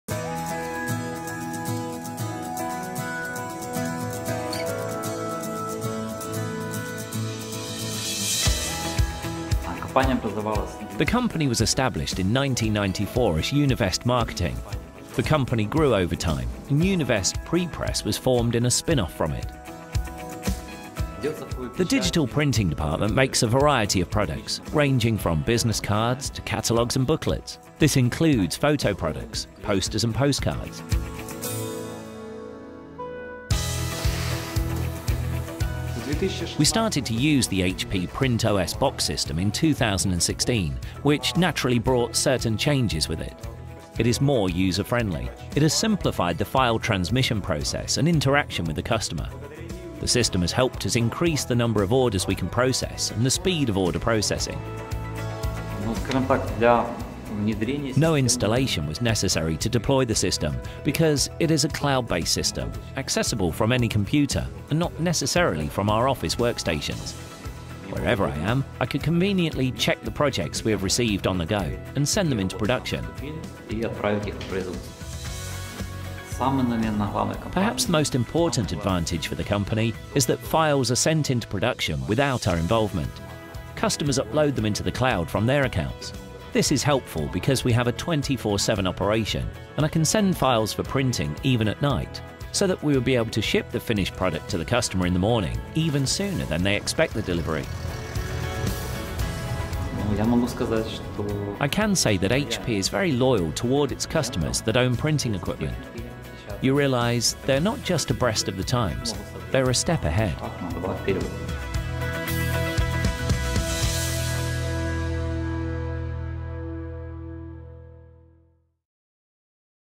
Univest Simplifies and Automates Production | HP Printos Box
Univest's digital print department makes a variety of products, ranging from business cards to catalogues and booklets. This includes photo products, posters and postcards. Univest started to use HP PrintOS Box in 2016, noticing immediately that it's more user-friendly, simplifying the file transmission process and interaction with customers and increasing the number of orders Univest can process.

PrintOS Box allows users to manage files captured from various sources, facilitate communication with print buyers and easily submit jobs to production.

Versatile digital presses with offset- and gravure- matching print quality for commercial and labels and packaging jobs